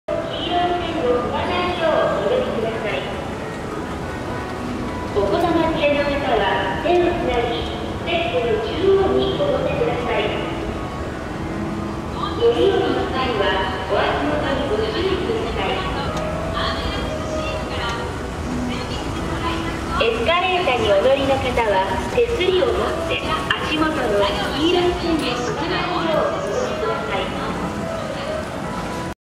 【フジテック製】ヤマダ電機 テックランド大津堅田店 昇りエスカレーター
ヤマダ電機 テックランド大津堅田店のエスカレーターです。